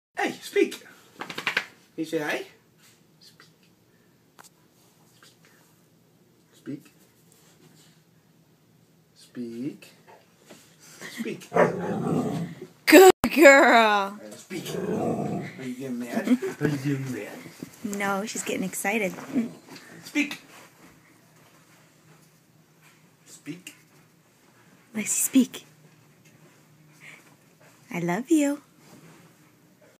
Rottie can talk!--so cute!
Lexie gets excited and starts talking